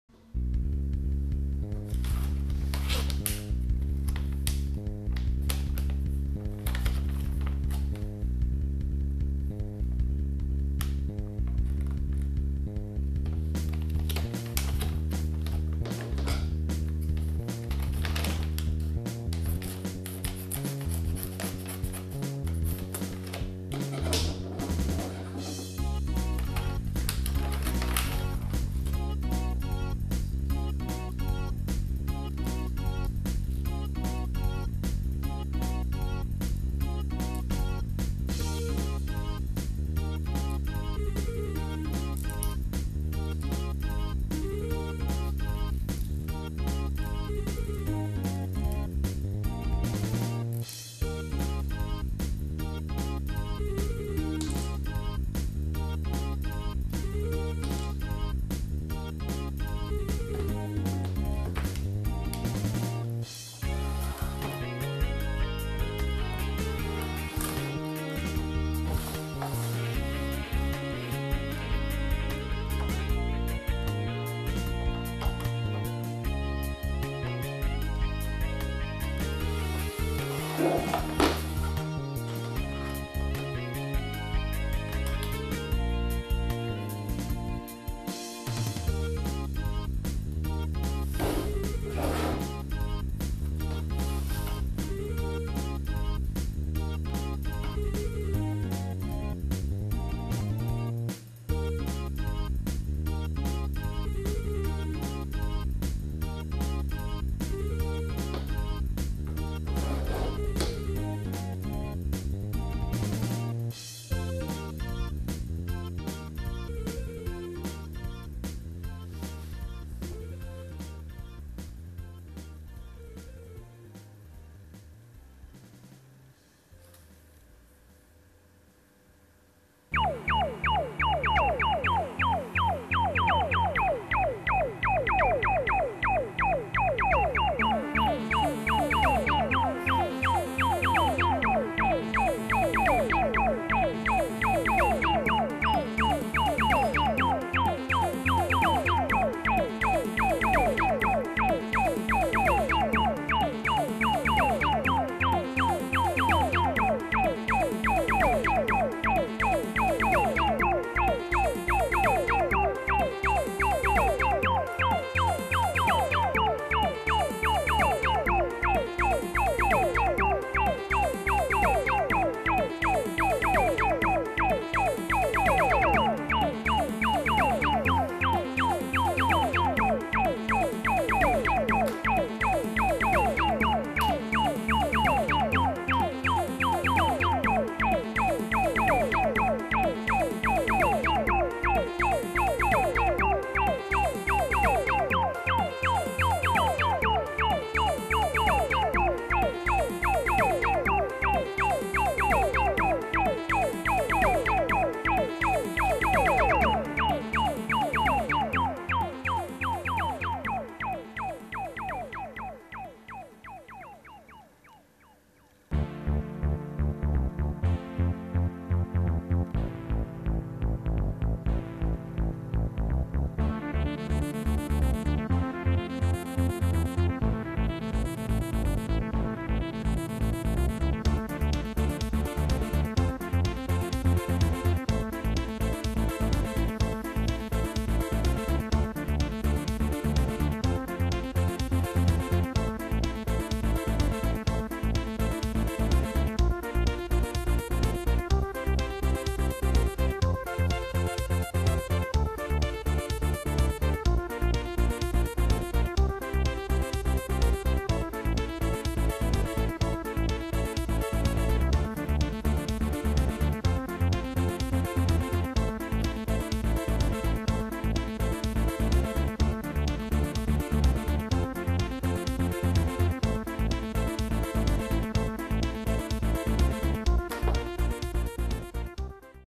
Unboxing and test of Sonorous HDMI cable in Full 3D HD
Unboxing and test of brand new HDMI black cable (bought for 3,80 EUR). Used for the test are LG 23EA63V-P computer display with IPS technology and Dell Latitude E5430 notebook computer with Windows 7 Professional. The test didn't go very well. The display didn't show proper picture, because it can't handle 1600x900 resolution.